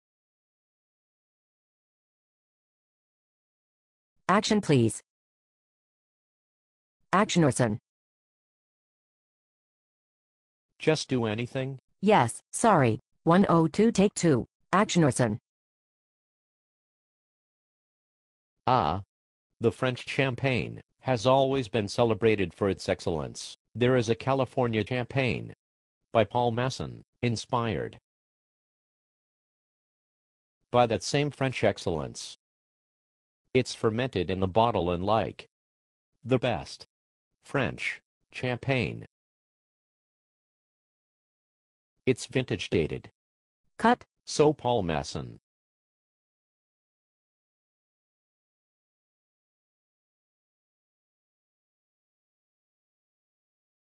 Orson Welles' Champaigne Commercial
Orson Welles shilling for Paul Masson.